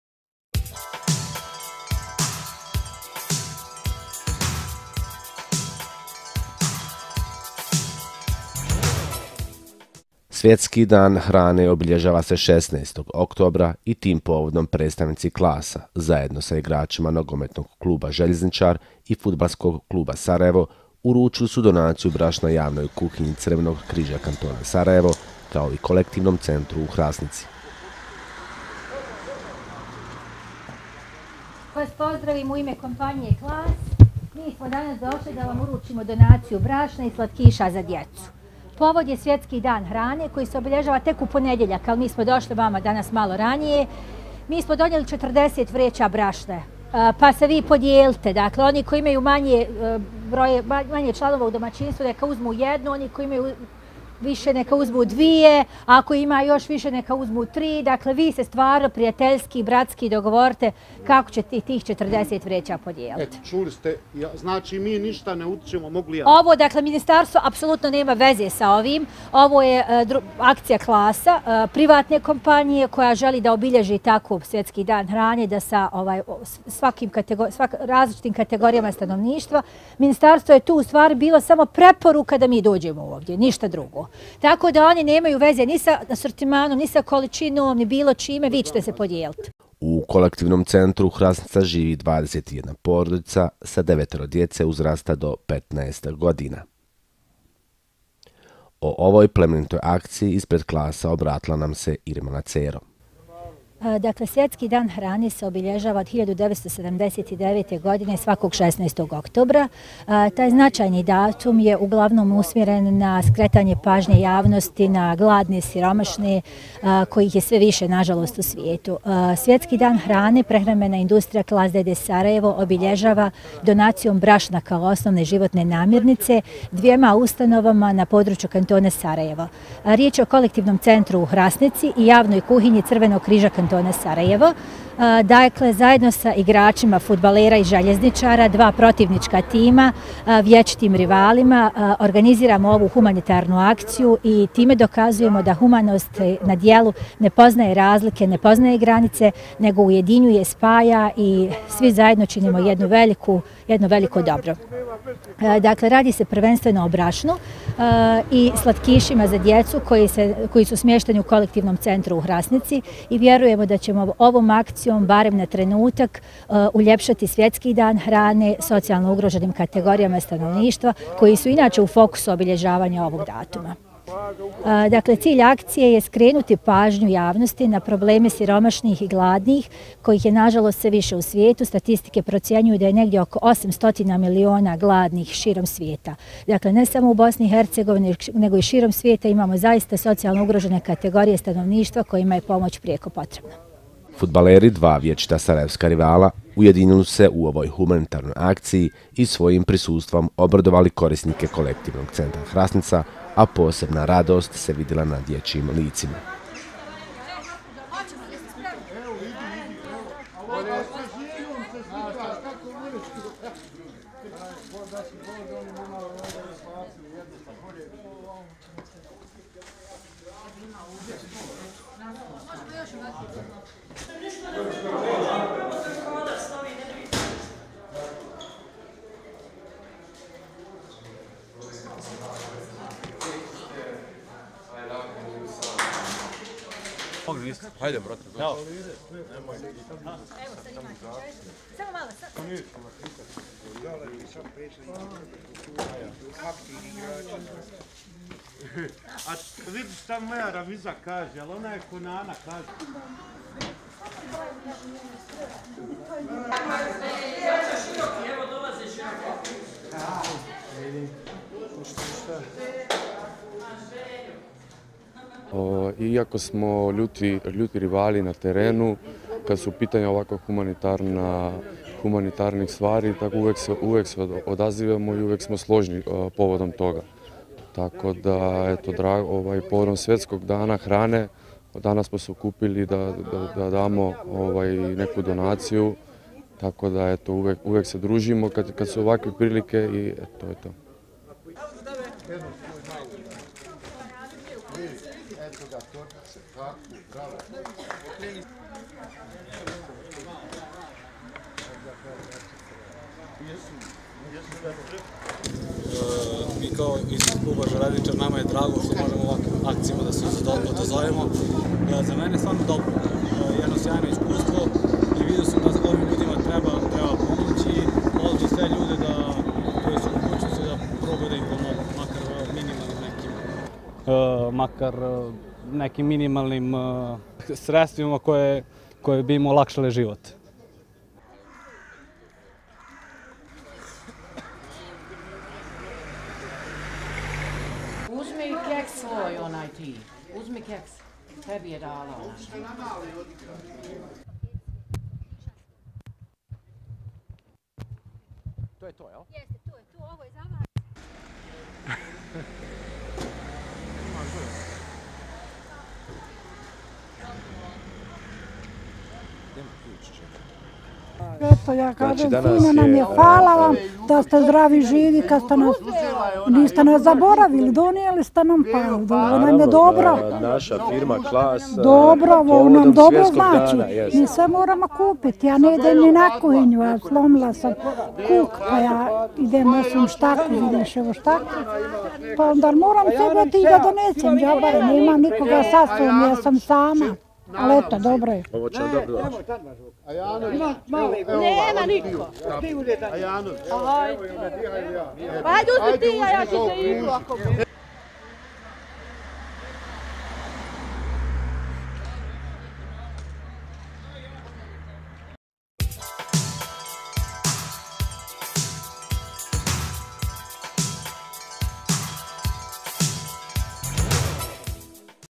SVJETSKI DAN HRANE: KLAS DONIRAO BRAŠNO - MTV IGMAN
Donaciju Klasa zajednički uručili fudbaleri Željezničara i Sarajeva.Povodom Svjetskog dana hrane, 16. oktobra, prehrambena industrija Klas d.d. Sarajevo donirala je dvije tone brašna Javnoj kuhinji Crvenog križa Kantona Sarajevo i Kolektivnom centru u Hrasnici. Donaciju su zajednički uručili Josip Projić, Kemal Osmanković, Mersudin Ahmetović i Krste Velkoski, igrači fudbalskih klubova Željezničar i Sarajevo, čime su pokazali složnost i ujedinjenost u ovoj društveno odgovornoj akciji.Javna kuhinja Crvenog križa KS dnevno za svoje korisnike priprema oko 1.650 obroka. U Kolektivnom centru u Hrasnici trenutno živi 21 porodica sa devetoro djece uzrasta do 15 godina. Korisnici obje ove ustanove žive u teškoj materijalnoj i finansijskoj situaciji i, kako kažu, donacija Klasa će im biti veoma značajna u predstojećem zimskom periodu.Inače, Svjetski dan hrane obilježava se od 1979. godine, a utemeljile su ga zemlje članice Organizacije Ujedinjenih nacija za prehranu i poljoprivredu.Cilj je, između ostaloga, podizanje svijesti o problemima gladi i siromaštva u svijetu, ali i o solidarnosti kao načinu njihovog rješavanja. .